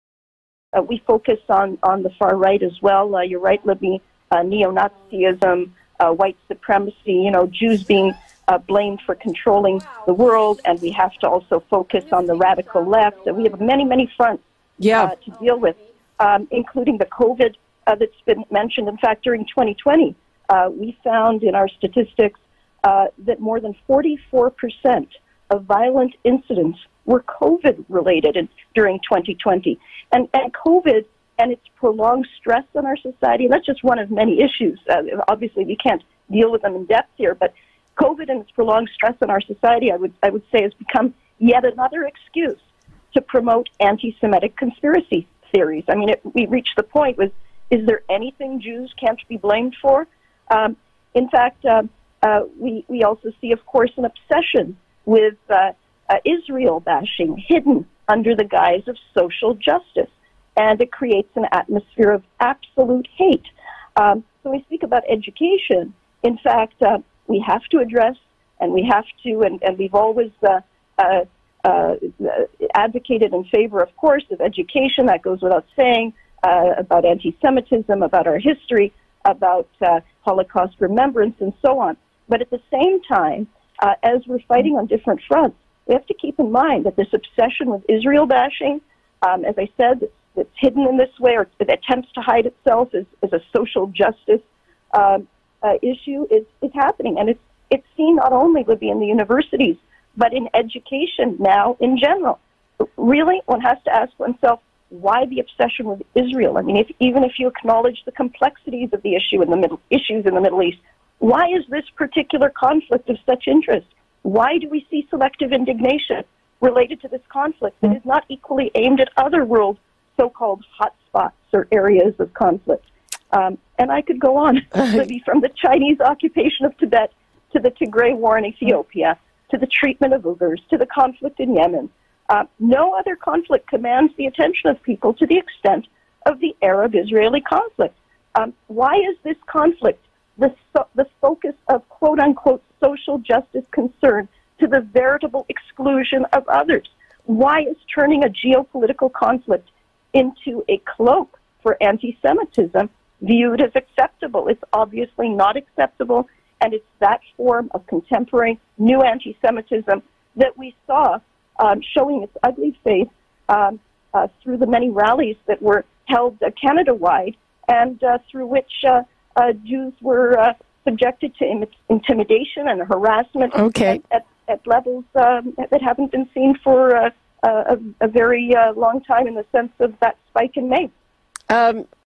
Interview: Contemporary Antisemitism in Canada
Libby Znaimer of Zoomer Radio interviews Michal Schlesinger, Senior Human Rights Liaison, League for Human Rights, B'nai Brith Canada and has a frank discussion about what contemporary antisemitism in Canada looks like.